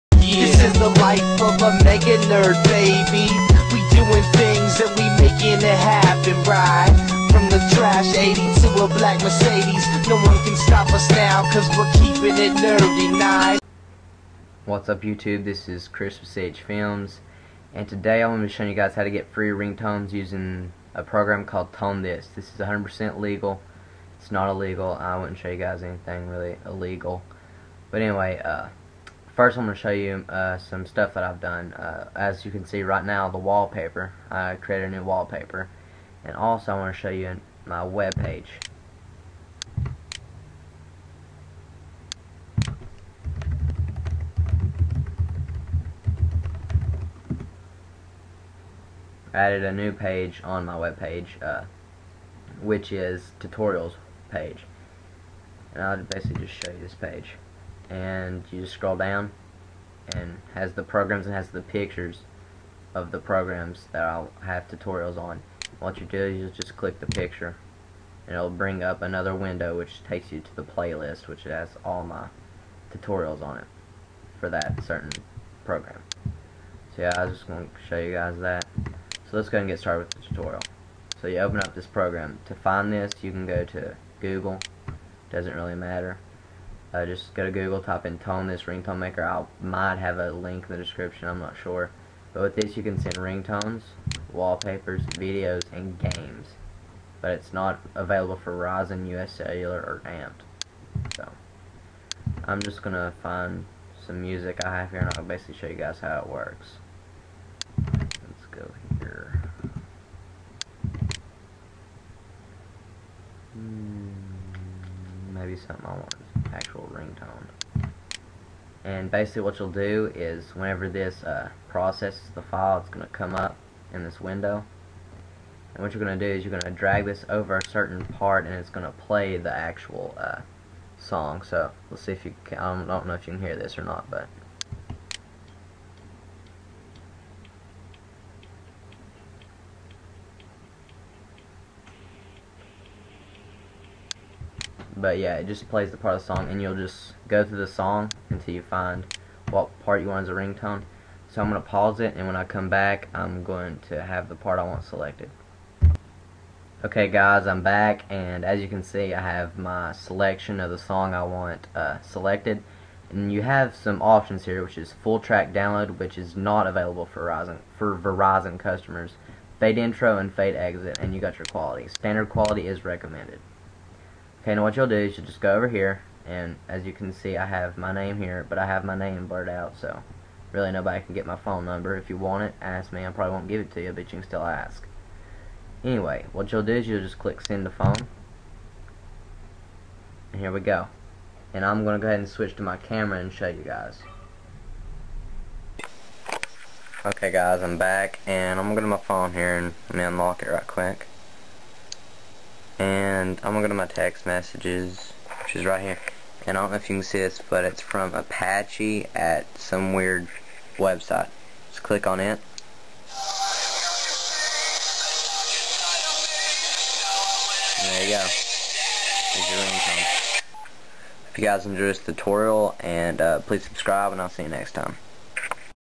How To Get Free Ringtones!!!
In This Video Tutorial I Will Be Showing You How To Get FREE Ringtones Using A Program Called ToneThis.